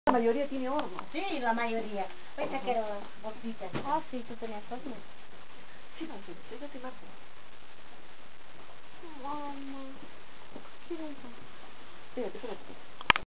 Tienda en Succha (Ancash - Perú)
Una tienda en las alturas de Ancash.